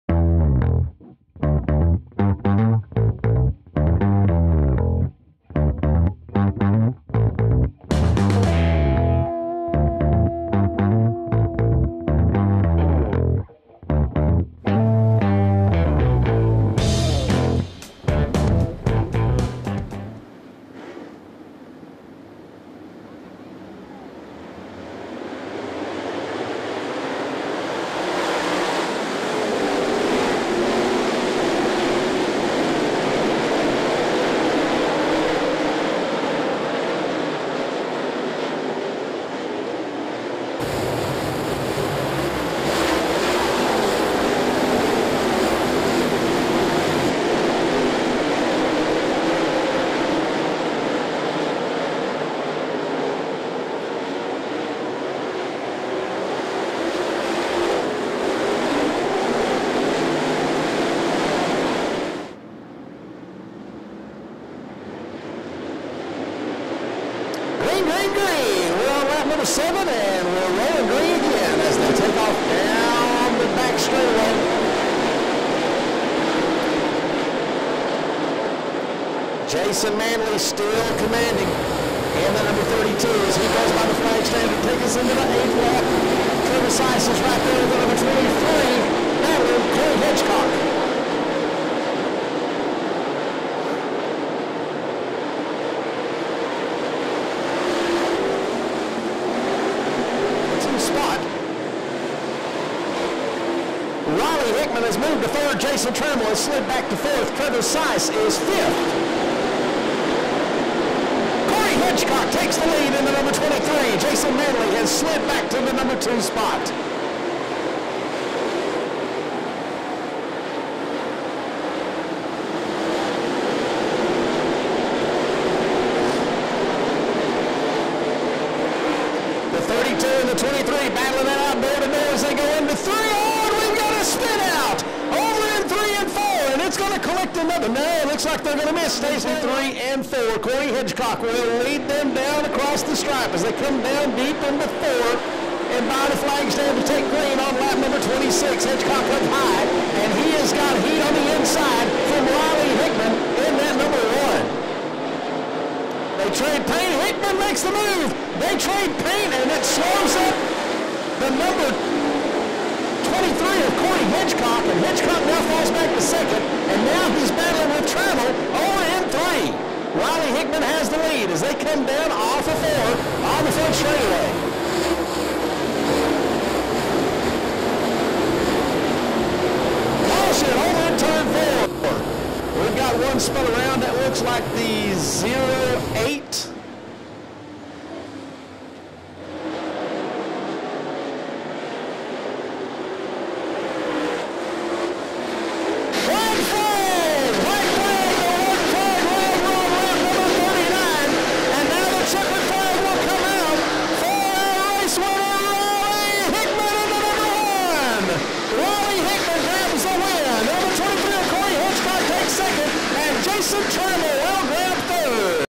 Volunteer Speedway Steel Head Late Models $2,000 to Win 4 18 15
Volunteer Speedway
Spring Thaw 4-18-15
Steel-Head Late Models $2,000 to Win

DVD $10